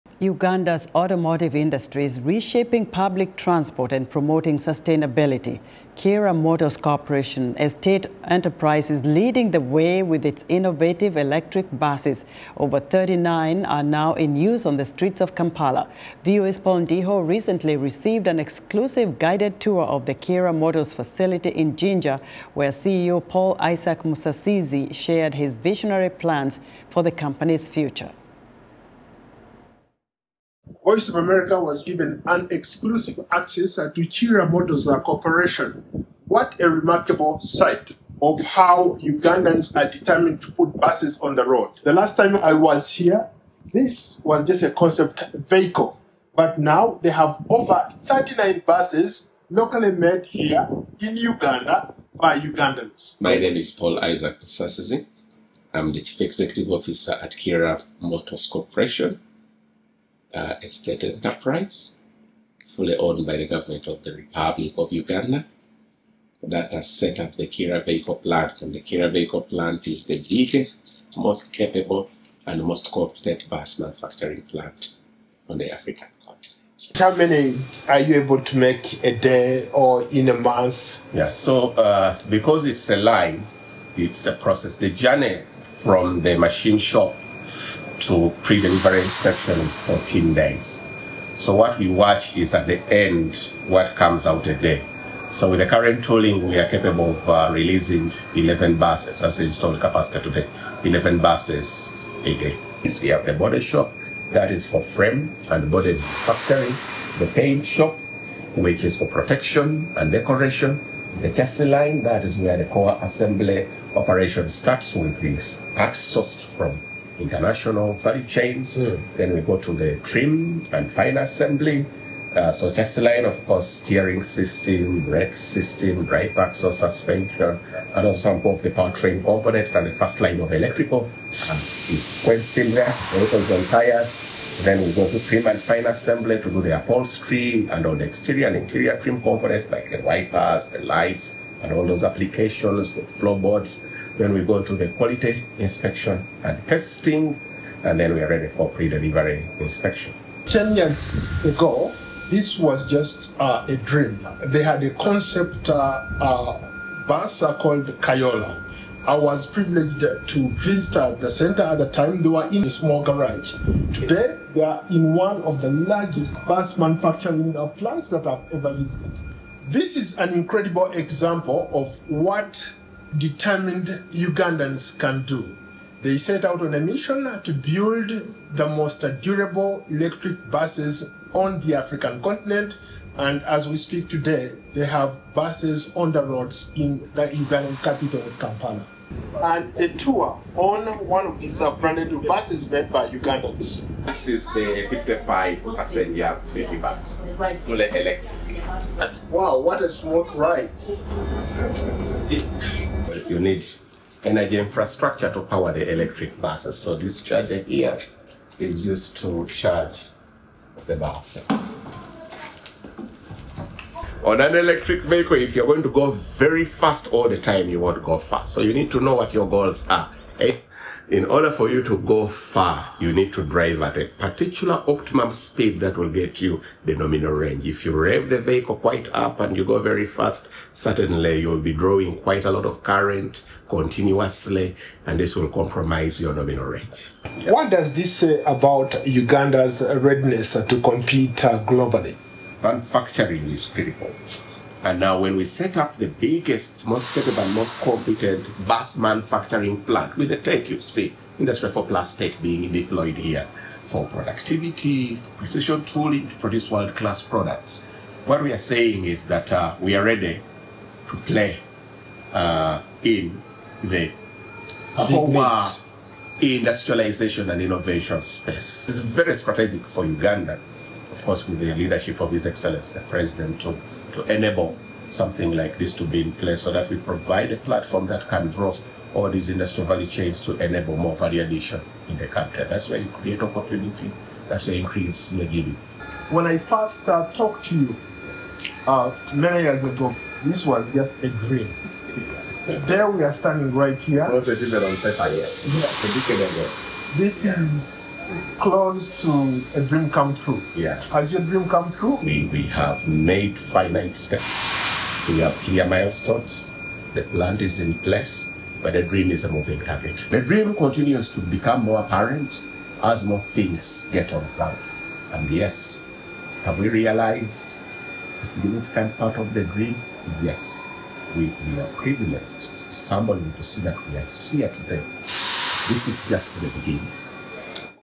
UGANDA'S KIIRA MOTORS RESHAPES PUBLIC TRANSPORT
Uganda's automotive industry is reshaping public transport and promoting sustainability. Kiira Motors Corporation, a state enterprise, is leading the way with its innovative electric buses, with over 39 of them navigating the streets of Kampala. VOA's Paul Ndiho recently received an exclusive guided tour of the Kiira Motors facility in Jinja, where CEO Paul Isaac Musasizi shared his visionary plans for the company's future. Industry analysts say Kiira Motors is not just creating jobs but contributing to wealth creation and setting a new standard for electric mobility in Africa.